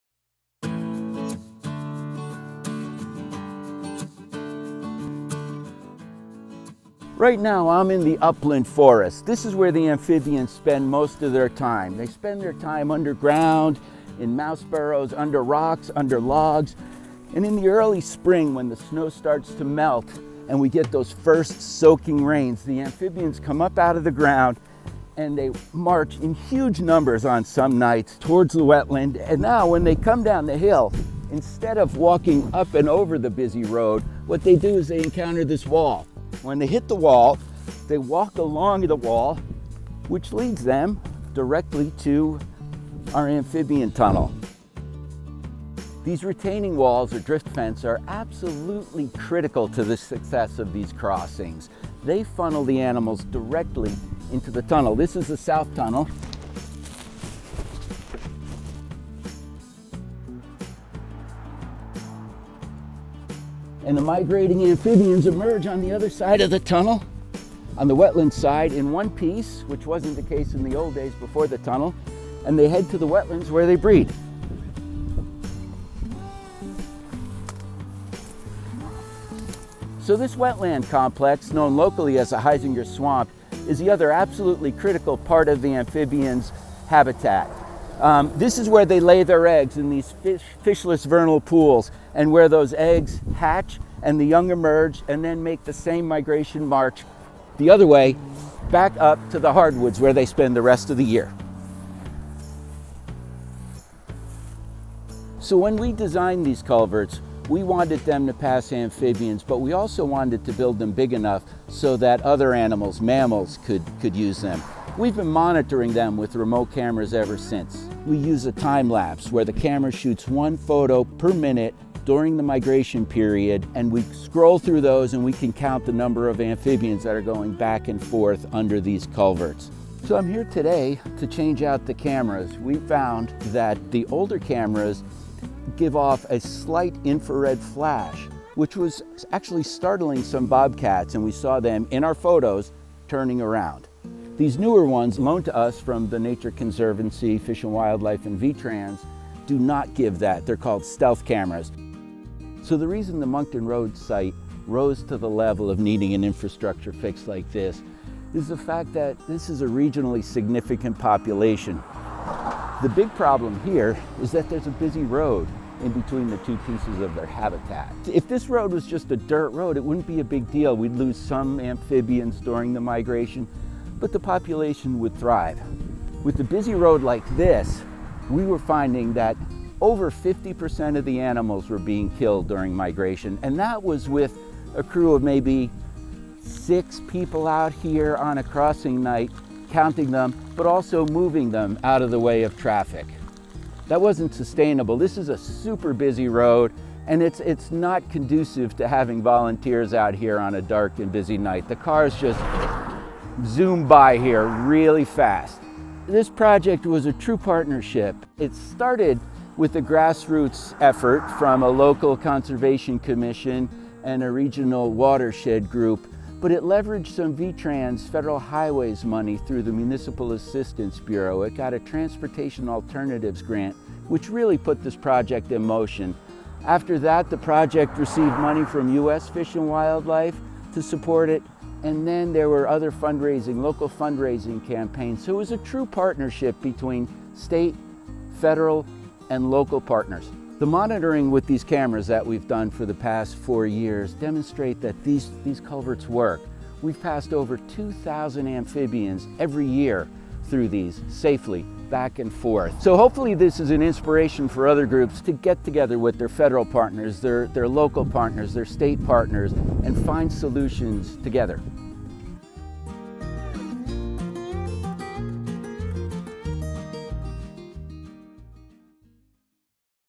Monkton Amphibian Crossing
Let Chris Slesar, VTrans Environmental Resources Coordinator, take you through a tour of one of our successful amphibian crossings in Monkton, VT.